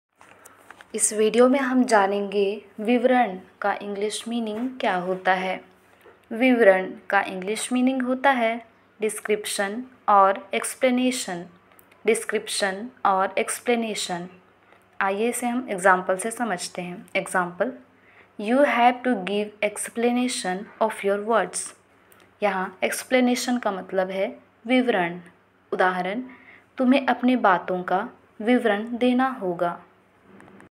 Vivaran ka english mein kya kahate hain | विवरण को इंग्लिश में क्या कहते हैं | Description meaning
Vivaran ko English me kya kahate hain
Vivaran को इंग्लिश में क्या कहते है

विवरण को इंग्लिश में description कहते हैं
Vivaran ko English mein description kahate hain